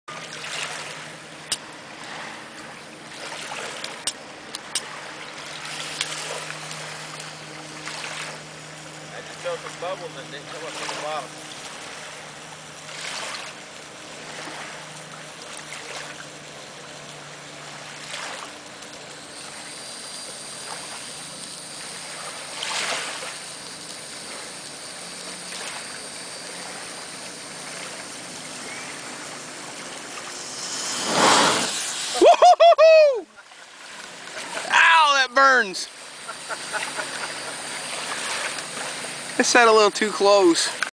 4th of july
practice golf ball packed with sparklers shouldve back up a little